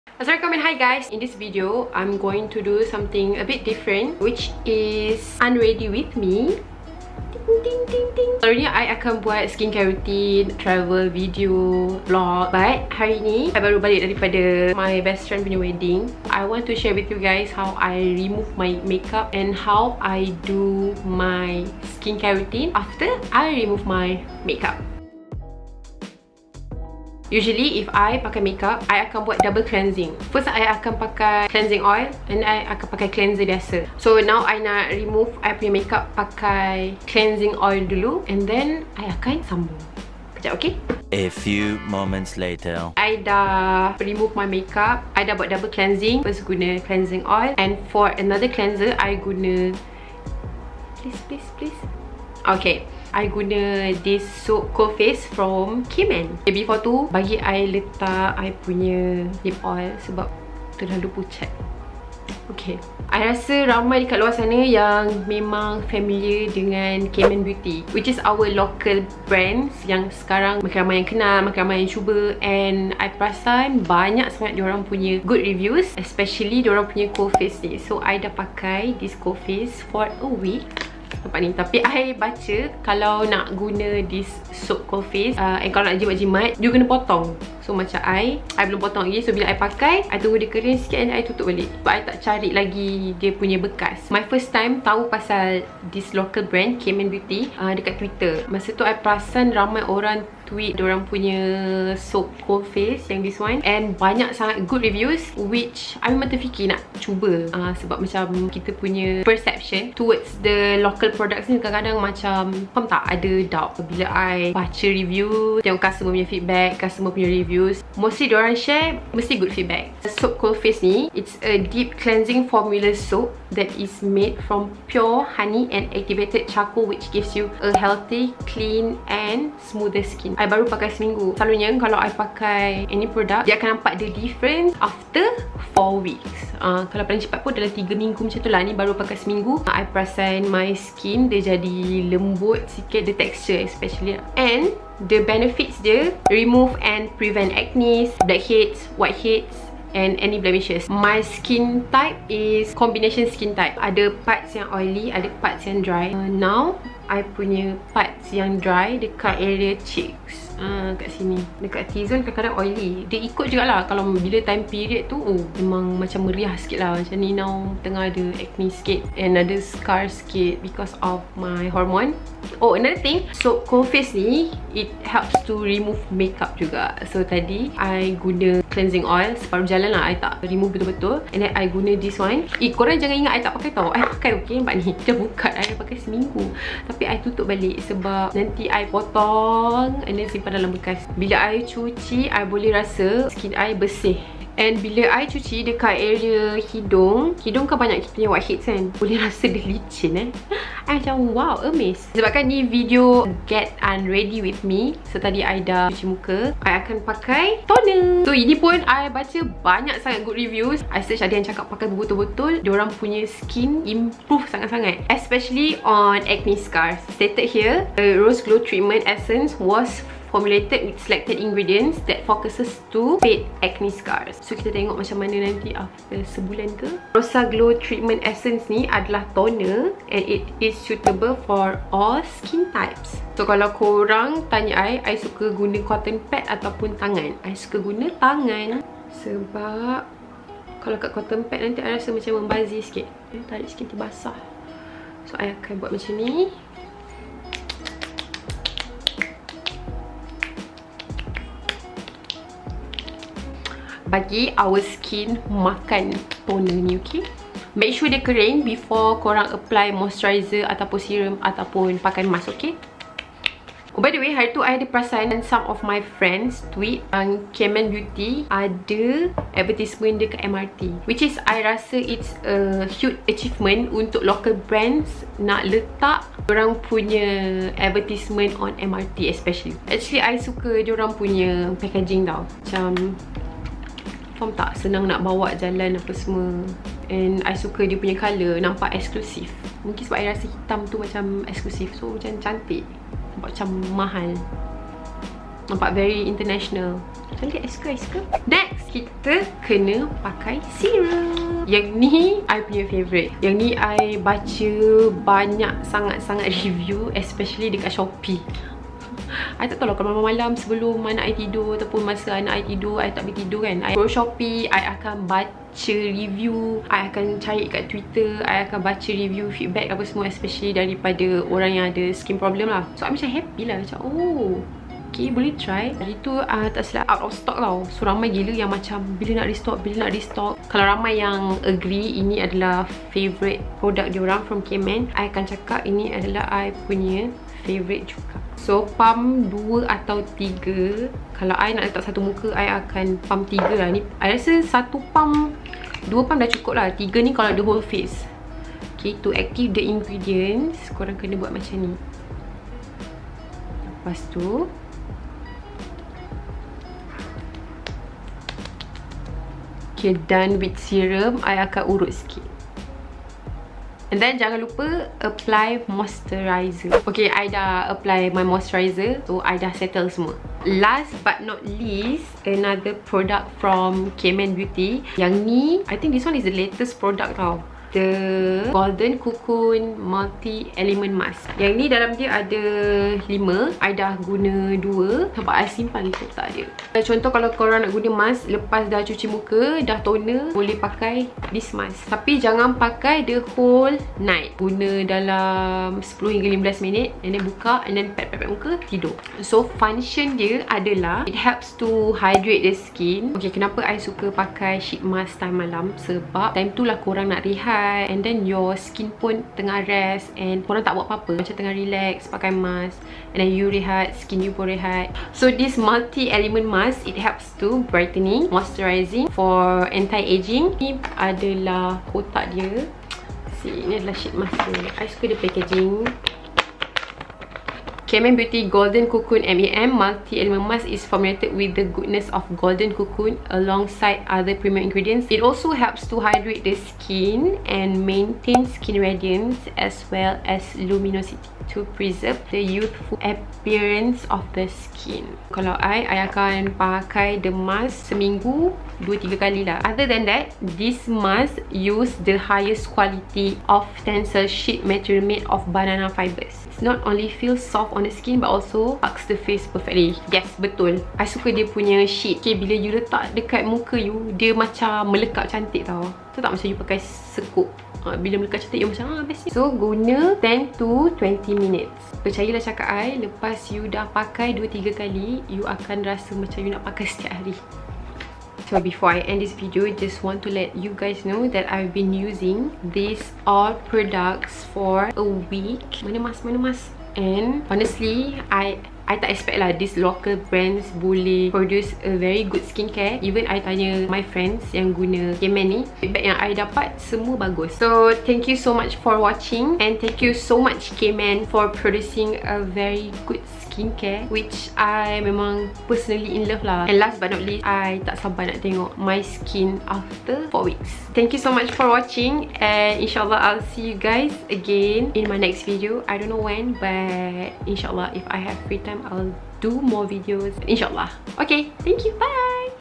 Get Unready with Me x Kayman Beauty
Salam and helu everyone! It’s always our job to make sure the skin is happy after we remove the makeup, agreed? I would NEVER skip my skincare routine steps! Especially when I found the products I love.

Products mentioned:

1. CoalFace Soap

2. Rosa Glow Treatment Essence

3. Skintella Repairing Serum

4. Golden Cocoon Multi Element Mask

Say hello to me at;